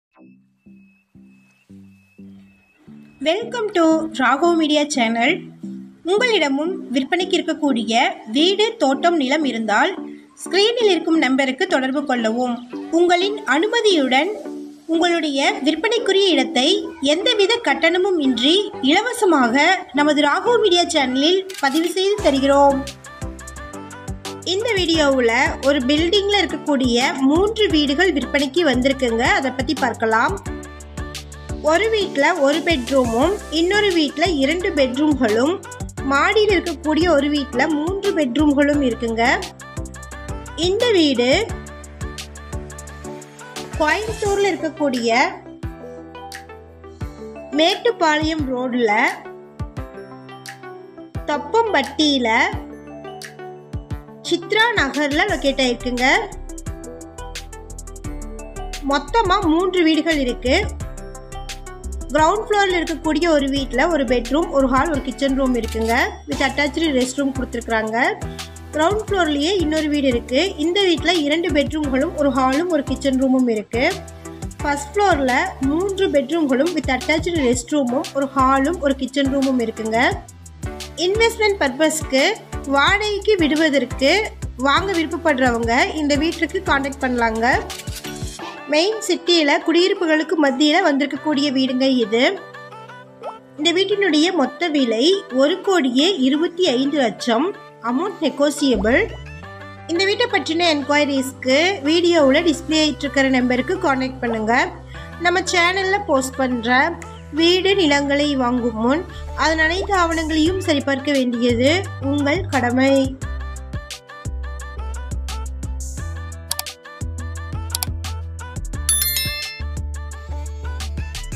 3 Houses For Sale in One Building, Rental Purpose House, Ragav Media Real Estate Properties
மேட்டுப்பாளையம் ரோடு தொப்பம்பட்டி சித்ரா நகர் வீடு உள்ளது மொத்தம் மூணு வீடு உள்ளது மாடியில் மூன்று பெட்ரூம் அட்டாச் பாத்ரூம் உள்ளது கீழே ஒரு வீடு ஒரு பெட்ரூம் உள்ளது இன்னொரு வீடு இரண்டு பெட்ரூம் உள்ளது வீட்டின் விலை 1. 25 கோடி பேசினால் விலை குறையும்

house design#3850x90
house design#
19architecturesmall
ToursReal Estate
money in real estateluxury
developmentlife of a real estate